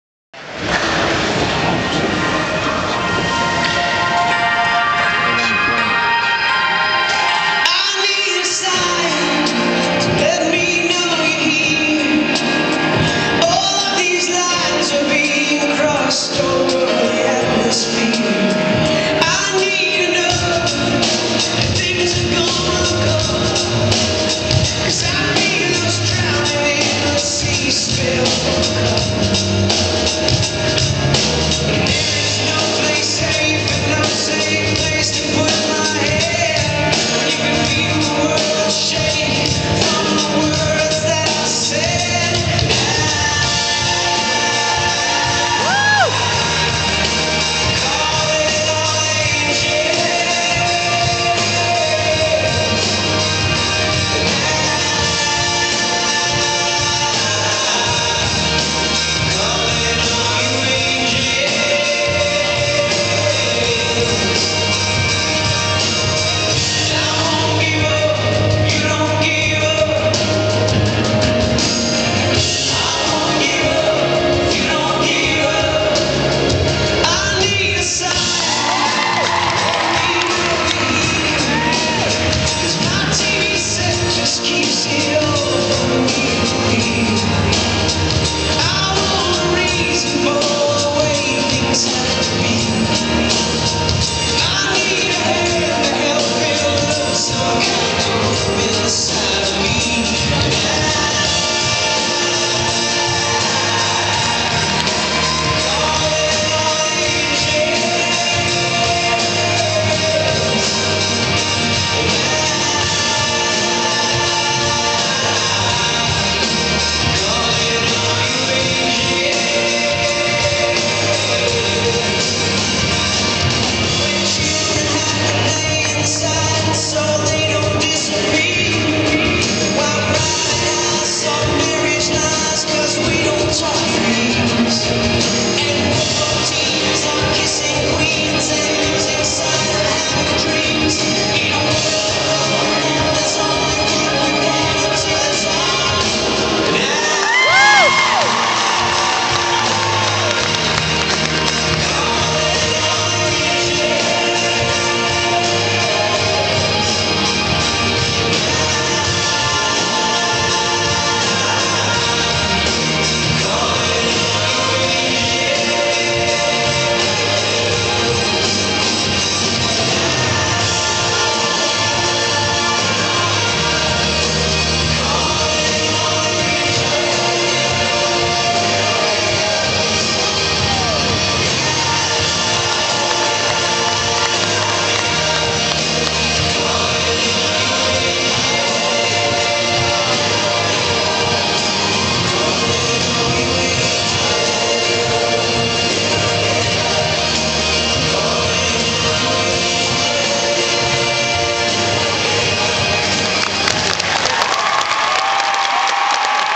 SMHS Winter Guard 2011
Performing at the drumline/winter guard exhibition 2011